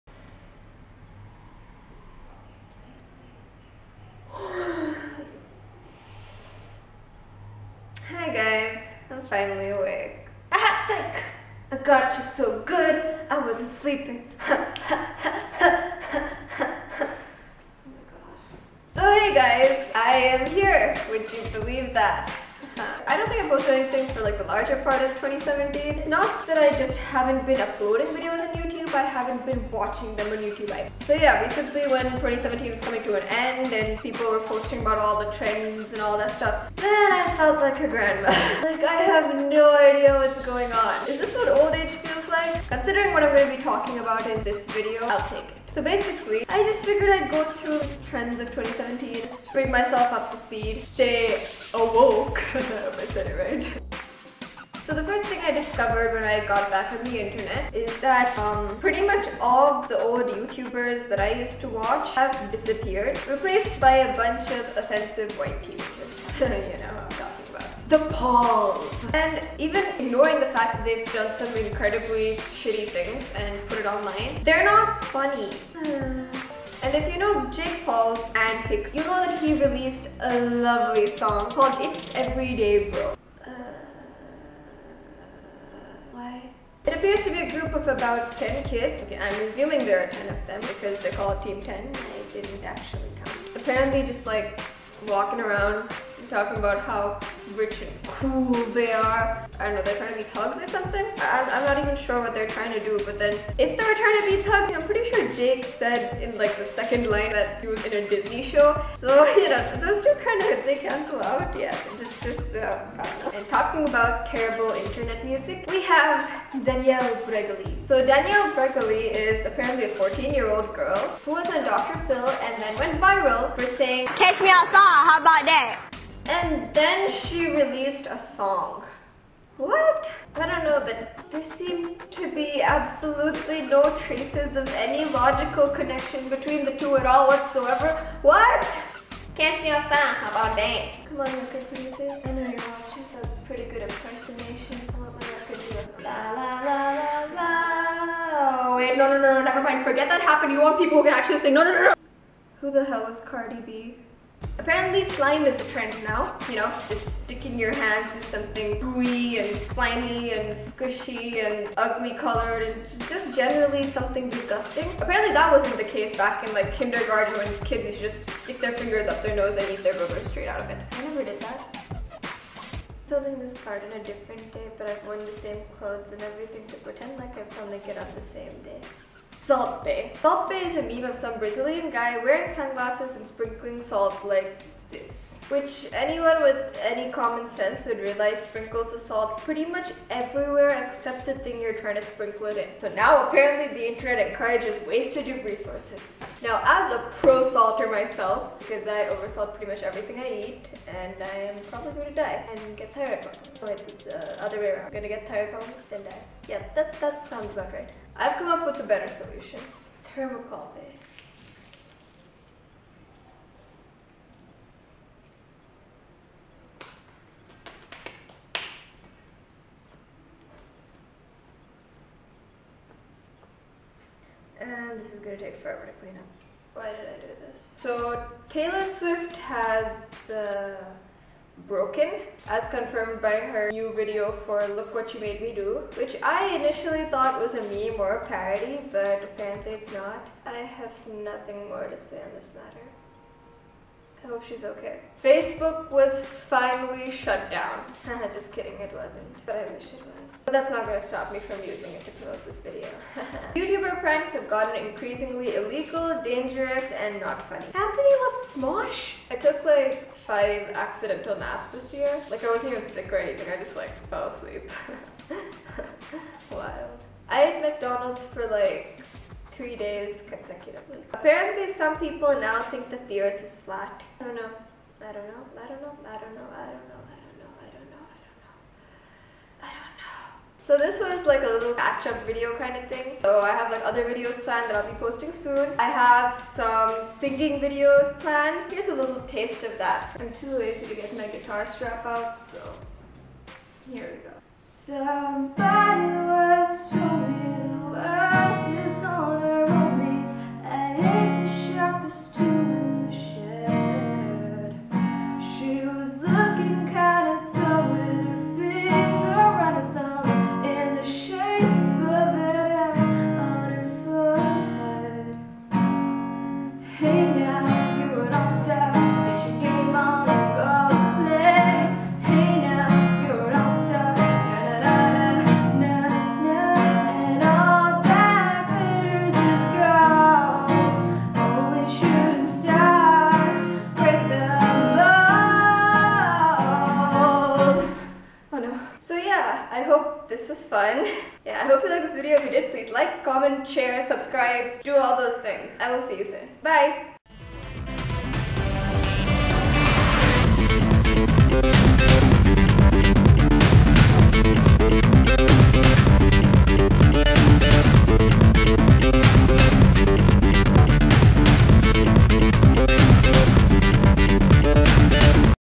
2017 IN A NUTSHELL
A recap of some of the super major important events of 2017

(sorry for over-editing)